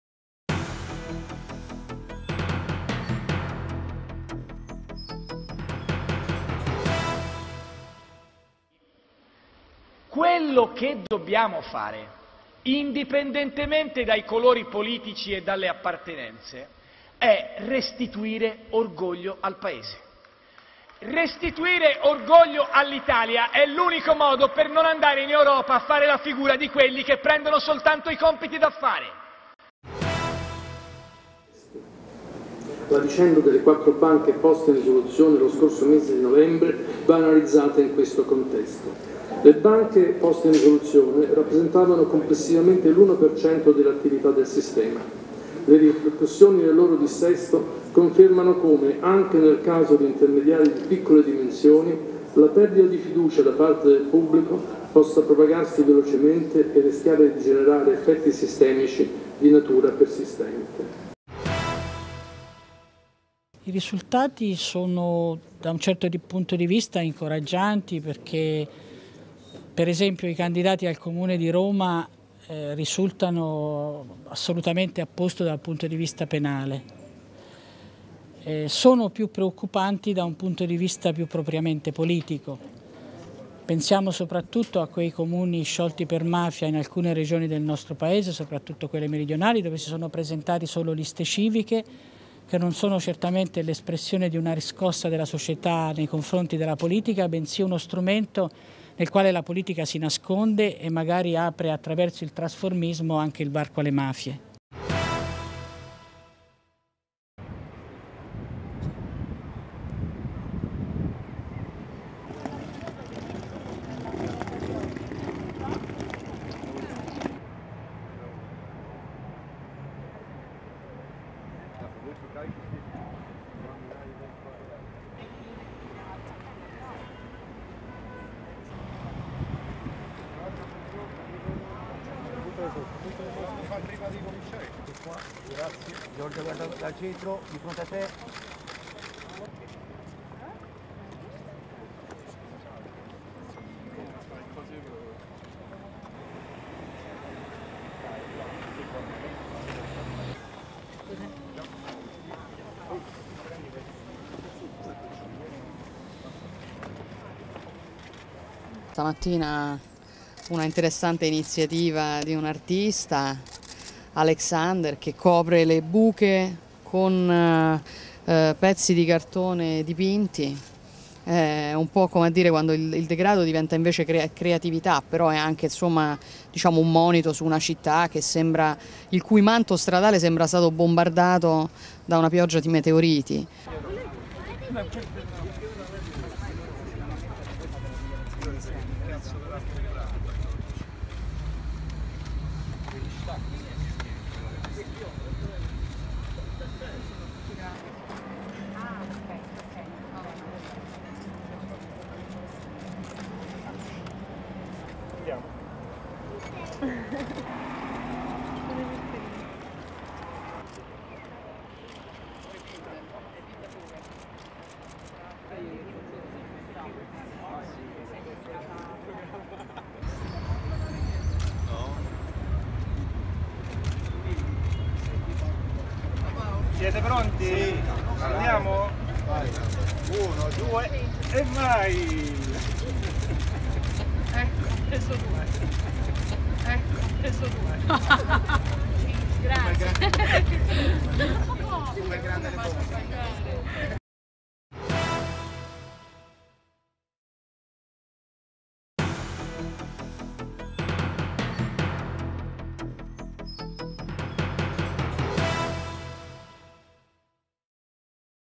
Tg Montecitorio La 31-05-16
(Agenzia VISTA) Roma, 31 maggio 2016
Renzi - Restituire orgoglio Italia in Ue
VIsco - Cirsi 4 banche fatto diminuire fiducia dei risparmiatori
Bindi - liste civiche possibile varco per la mafia
"L'Arte riempie le buche" - la performance al Colosseo